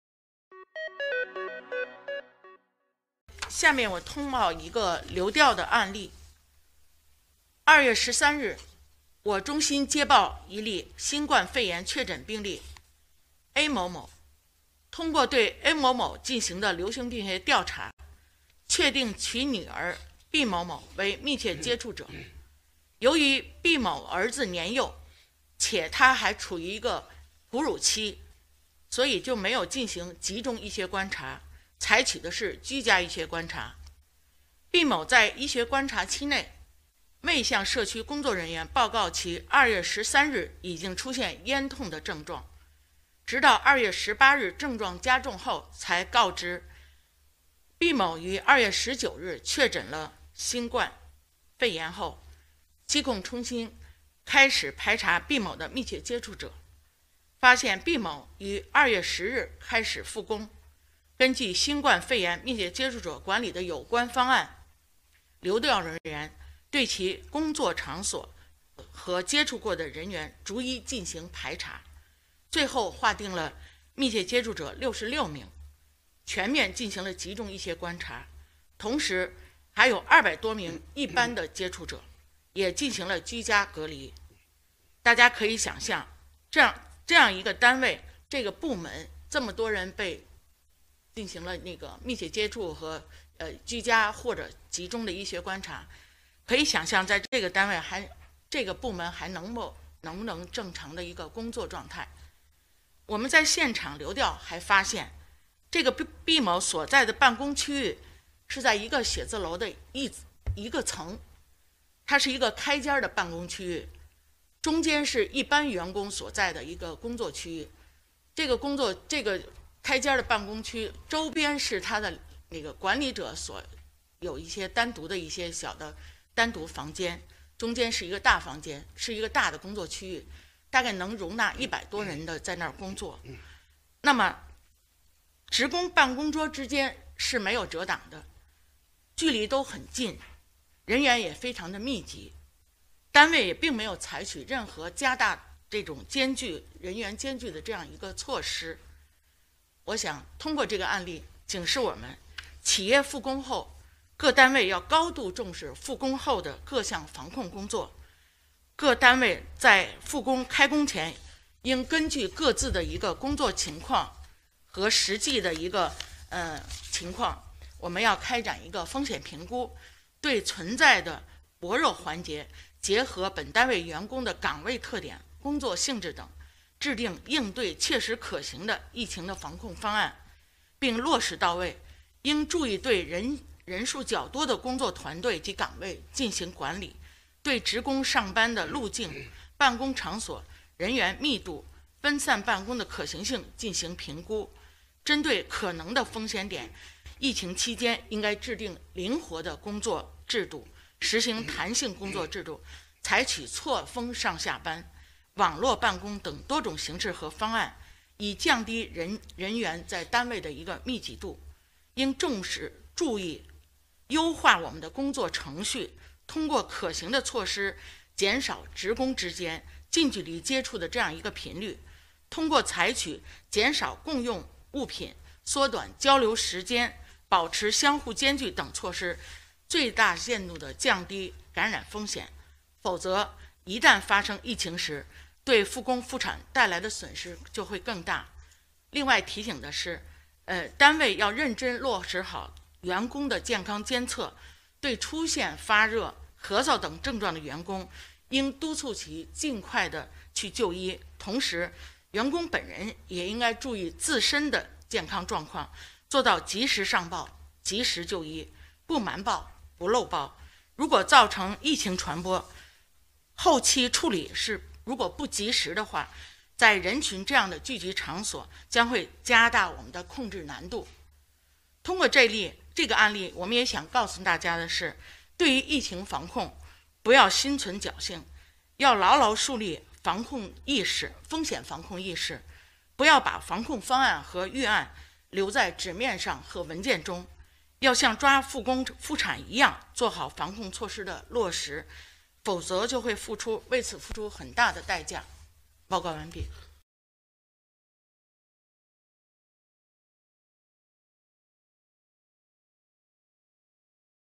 一员工复工9天后确诊，66人被隔离
2月21日，北京市疾控中心副主任庞星火介绍了一起新冠肺炎病例复工9天后发病导致全公司隔离的案例。庞星火提醒，对于疫情防控不要心存侥幸，要像抓复工复产一样做好防控措施的落实，否则就会为此付出代价。